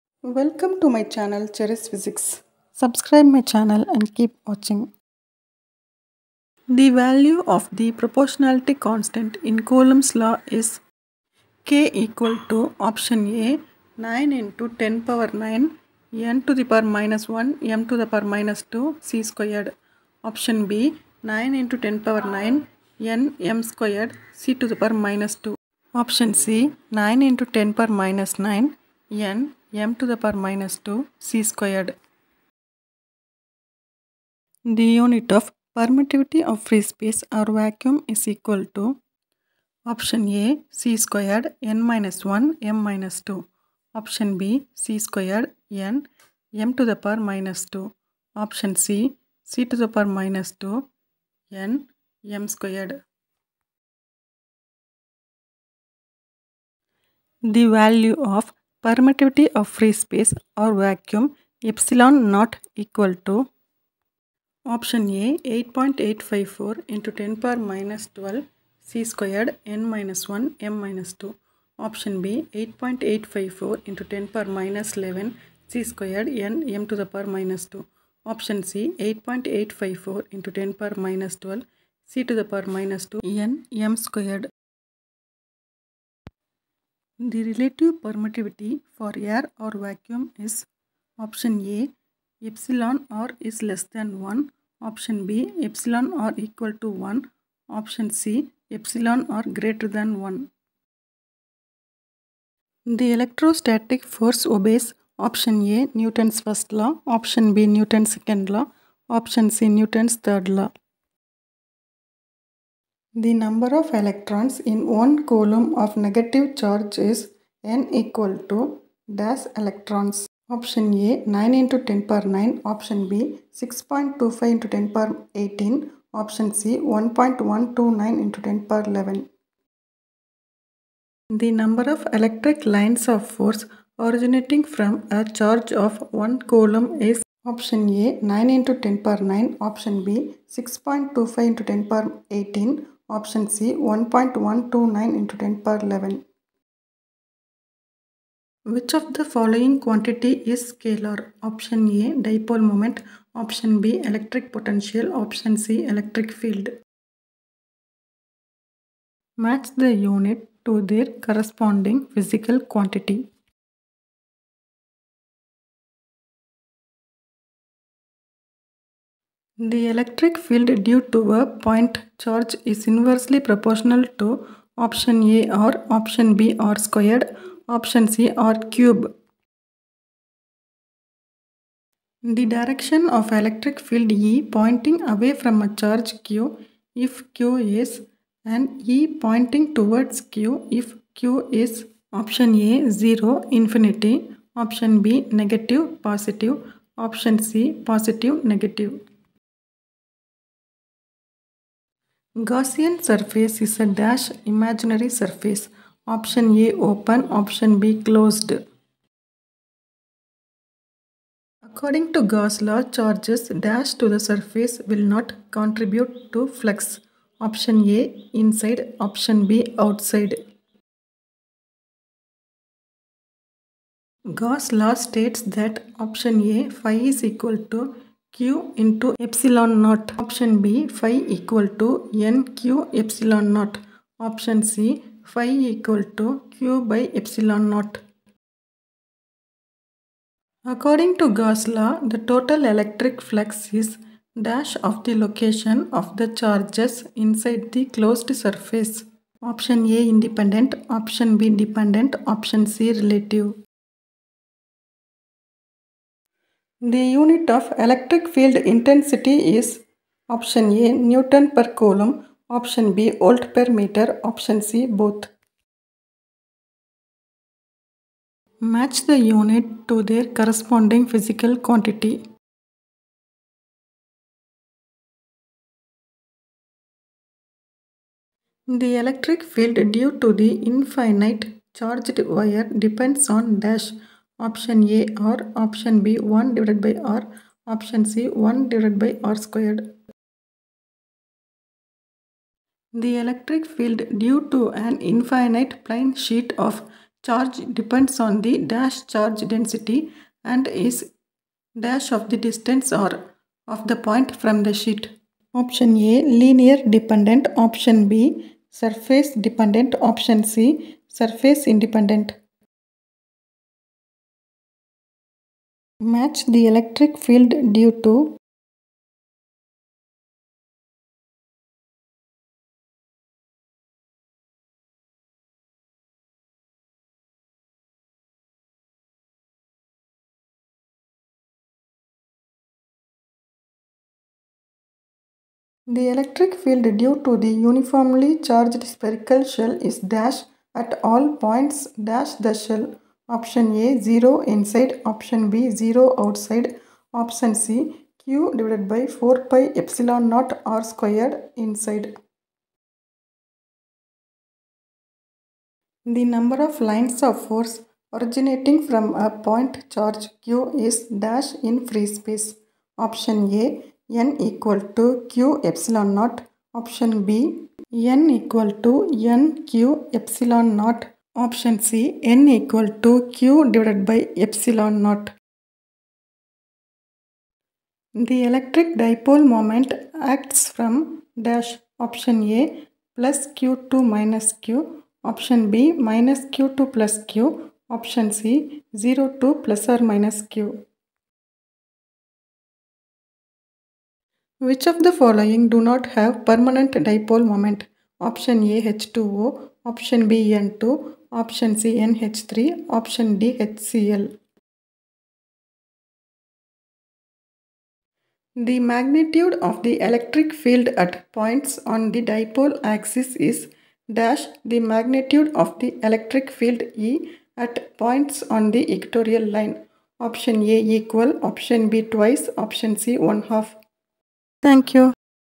💥 UG TRB PHYSICS 💥 EXAM 2023 💥 IMPORTANT 25/25 MCQ'S ☑️👍 UNIT 4 ELECTRICITY 💥 SGT EXAM 2023 💥
6. Given : q = 1 C , e = 1.6 x 10 -¹⁹ C

q = n e

⇒ n = q / e

        = 1 ÷ ( 1.6 x 10 -¹⁹ )

    n = 6.25 x 10 ¹⁸

7.  Given : q = 1 C ,

ε 0 = 8.854 x 10 -¹² C² N -¹ m -²

N = q / ε0

    = 1 ÷ ( 8.854 x 10 -¹²)

 N = 1.129 x 10 ¹¹